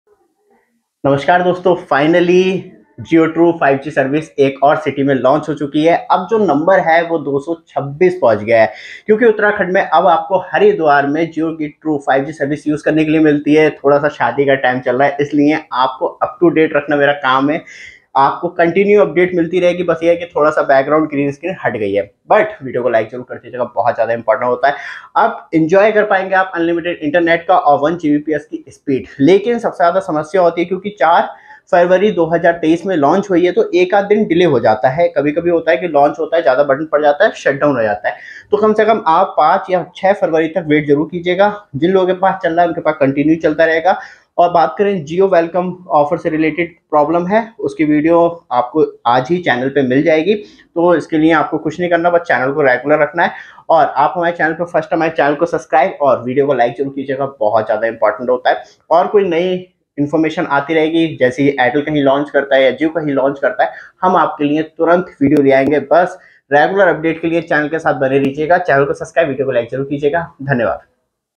Jio True5G Launched in Haridwar | Jio Welcome Offer Details
Jio True5G has been launched in Haridwar with Jio True5G Unlimited data With 1Gbps